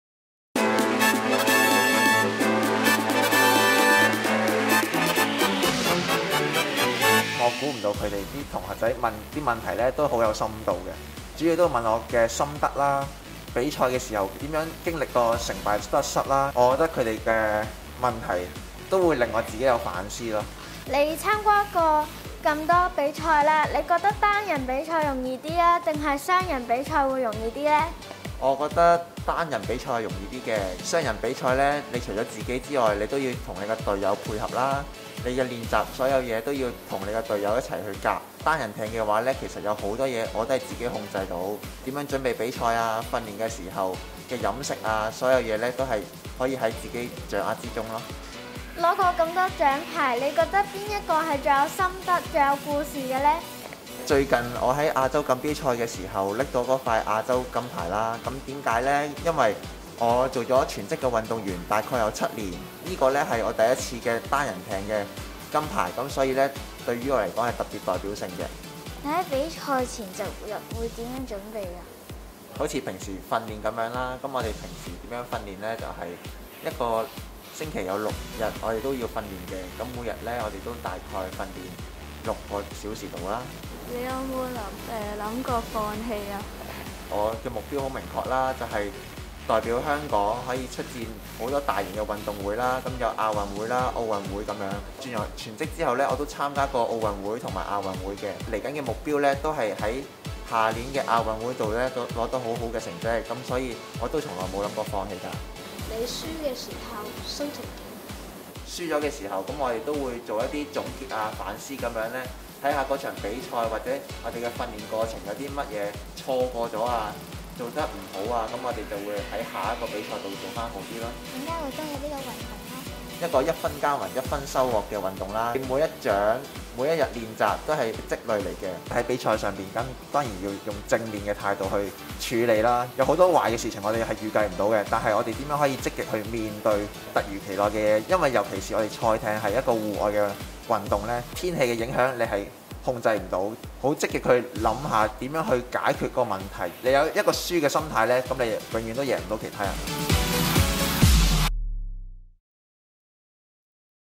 動感校園2022-2023📚｜天主教佑華小學🗳️｜受訪運動員：趙顯臻🚣🏻‍♂️｜體路✖精英運動員慈善基金賽馬會動感校園小記者培訓計劃
「精英運動員慈善基金賽馬會動感校園小記者培訓計劃」內容包括新聞寫作、體育攝影及新聞攝錄技巧課堂，由現役傳媒人、專業攝影師授課，參與學校表現優秀的學生將有機會跟隨《體路 Sportsroad》專業記者一同採訪香港體育賽事，訪問香港精英運動員，做個真正小記者！